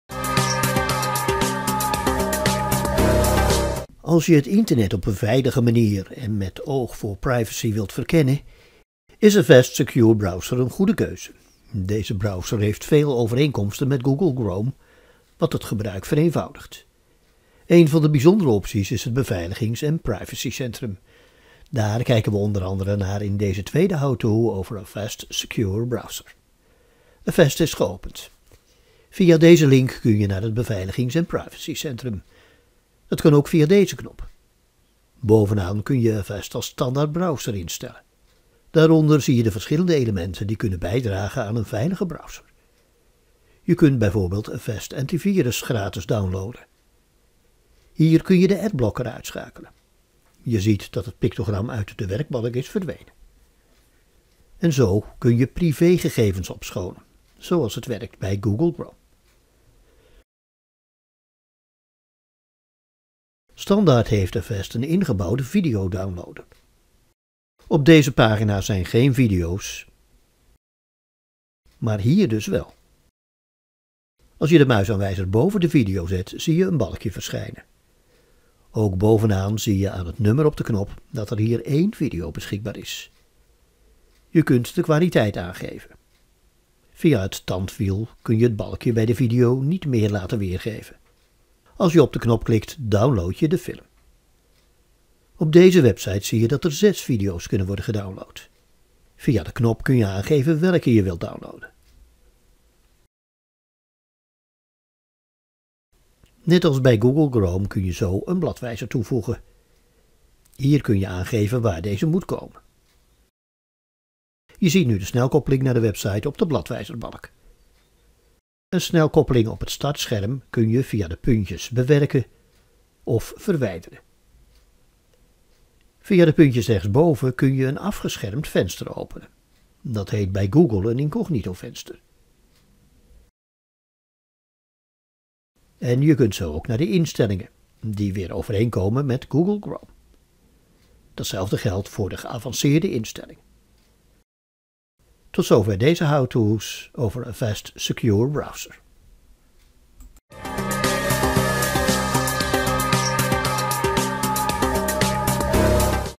Avast Secure Browser: beveiligings- en privacycentrum
Als je het internet op een veilige manier en met oog op je privacy wilt verkennen, is Avast Secure Browser een goede keuze. Deze browser heeft veel overeenkomsten met Google Chrome, wat het gebruik vereenvoudigt. Een van de bijzondere opties is het beveiligings- en privacycentrum. Daar werpen we in deze how to dan ook een blik op.